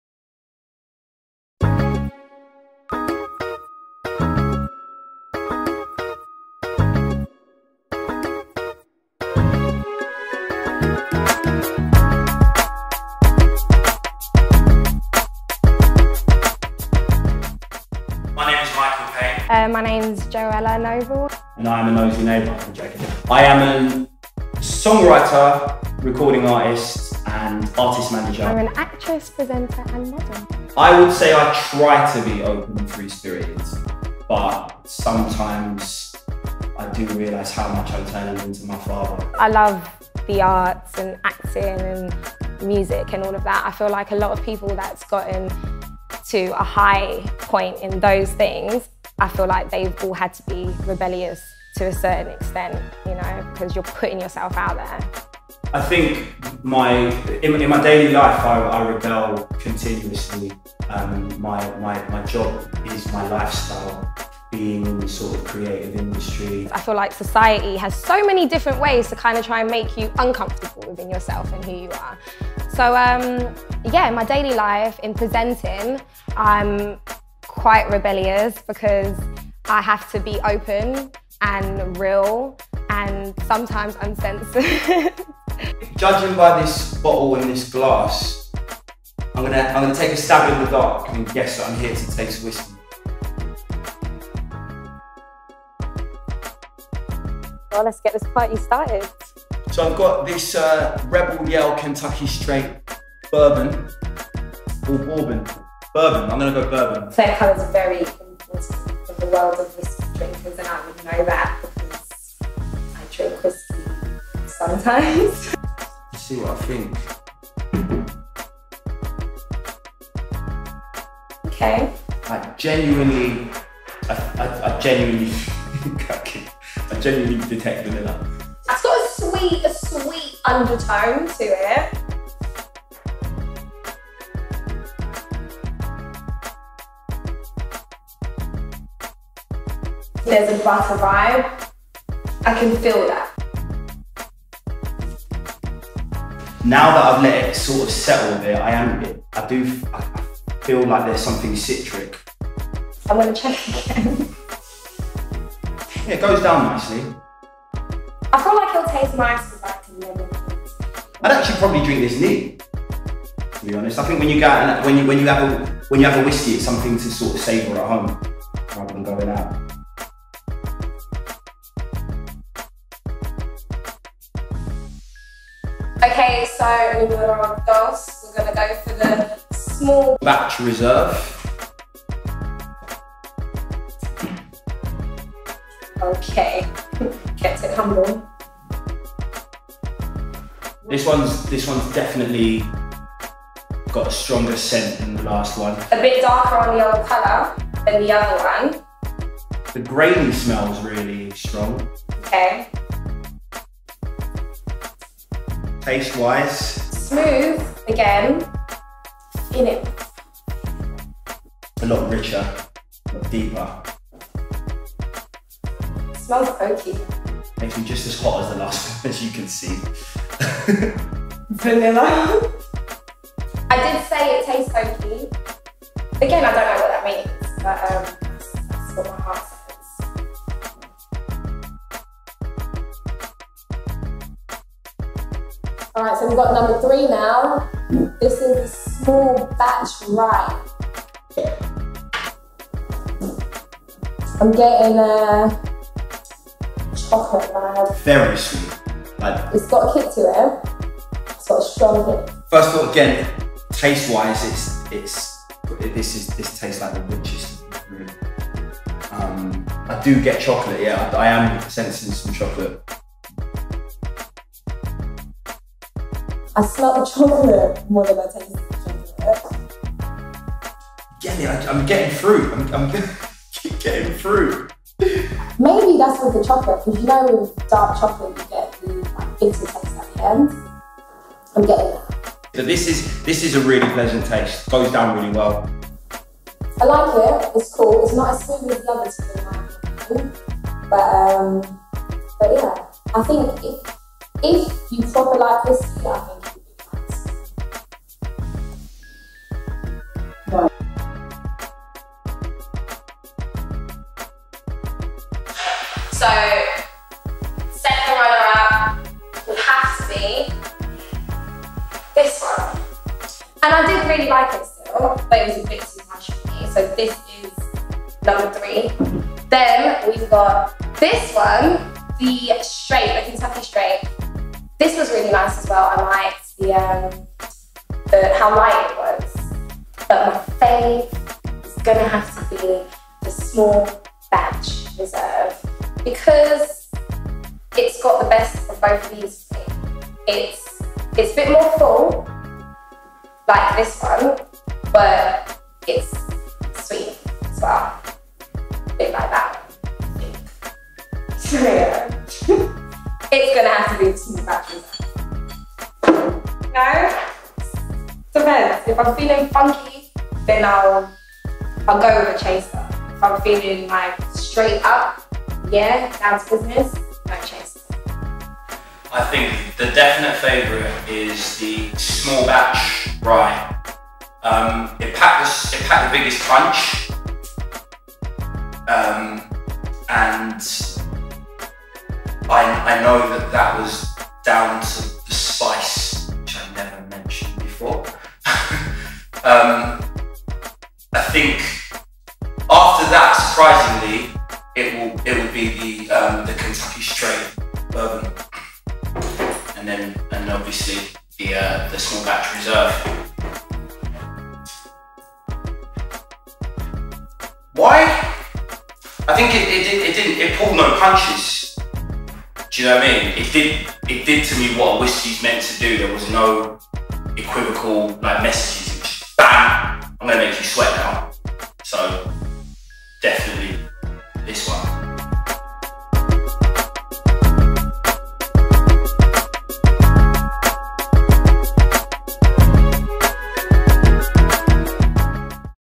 Rebel Yell Whiskey Tasting | Joelah Noble & Mikill Pane
Mikill Pane & Joelah Noble talk about their rebellious natures during a whisky tasting with Rebel Yell Whiskey.